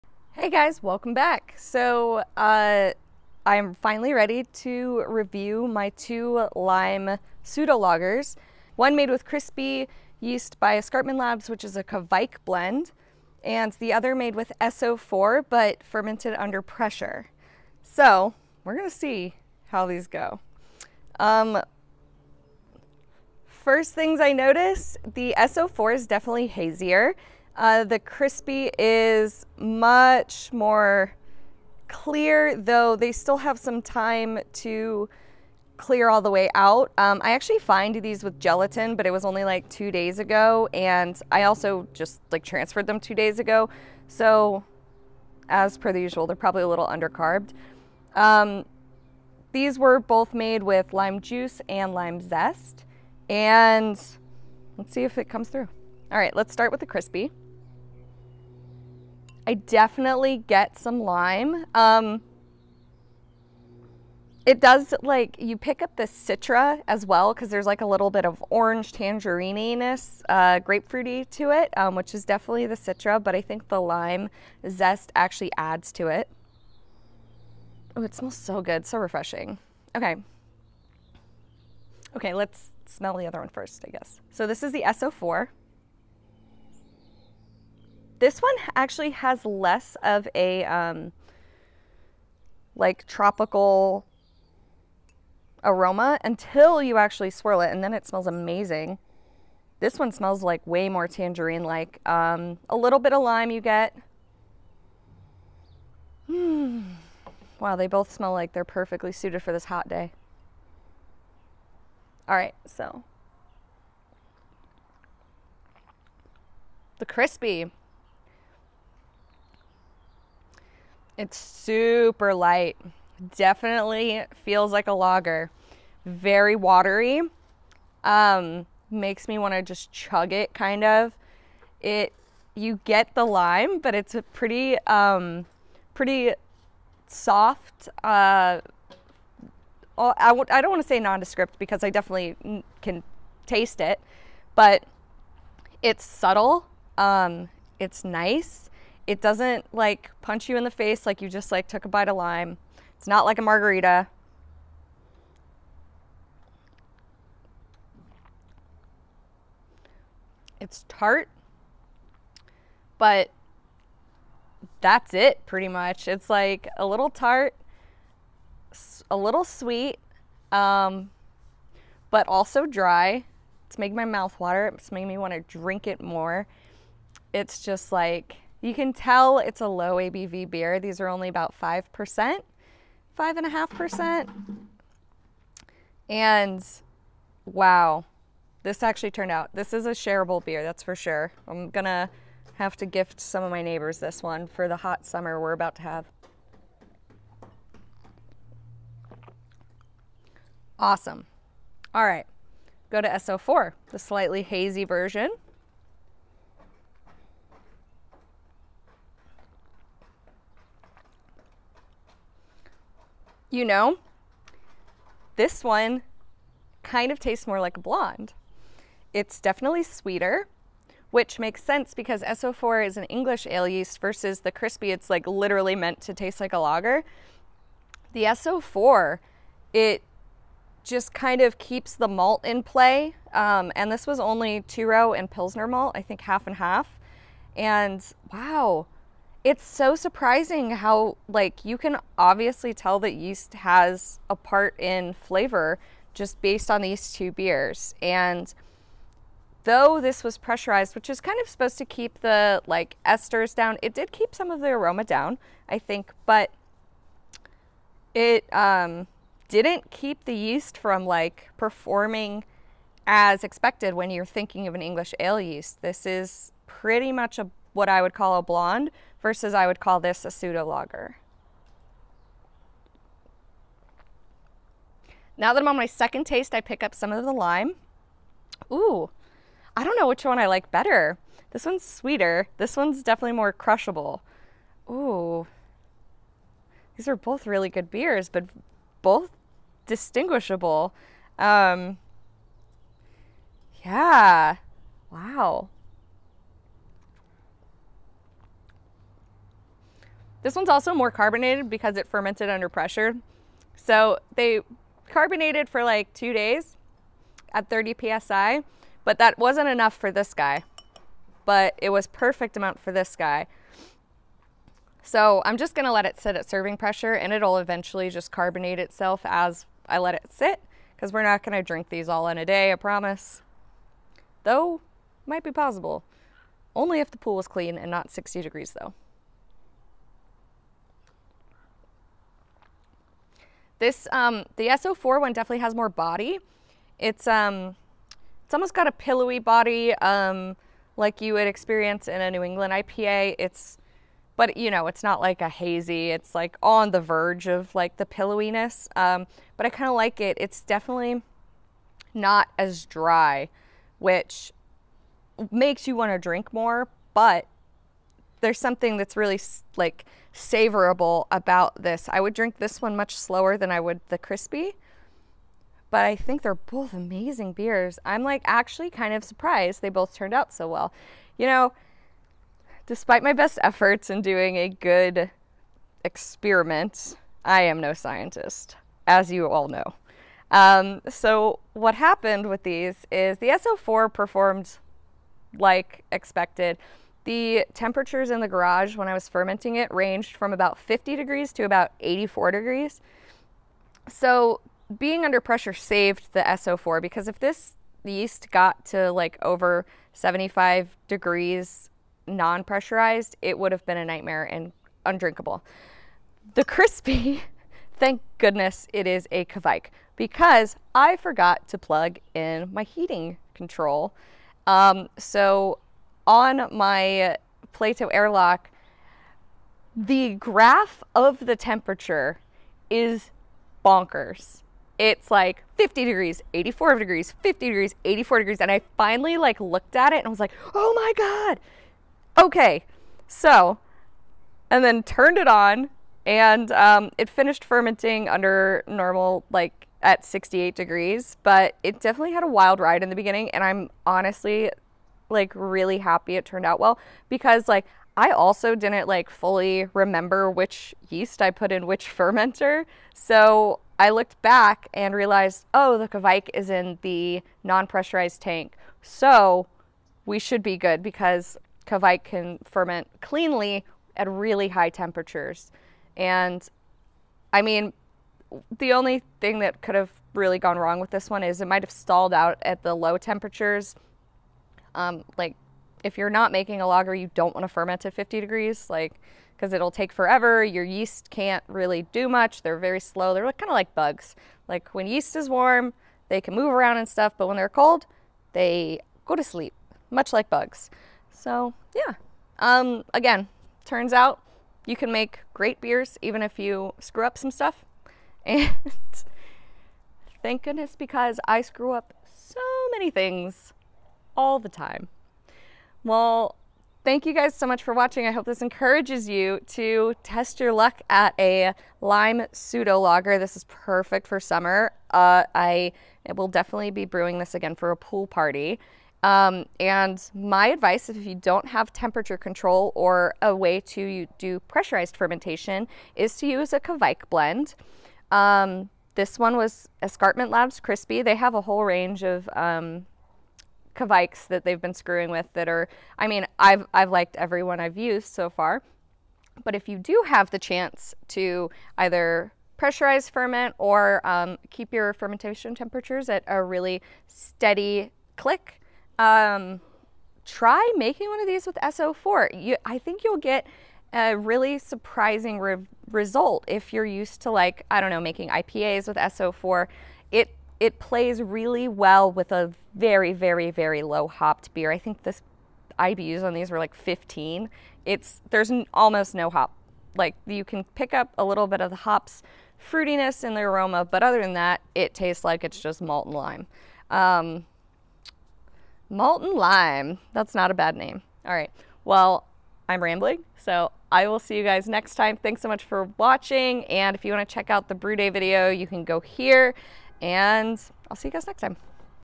Lime Pseudo Lager Review (S-04 Yeast VS. Escarpment Lab's Krispy Kveik Yeast)
Well I didn't expect these to both turn out amazing but I'm wrong about a lot of things! *Recipe & Brewday video in description*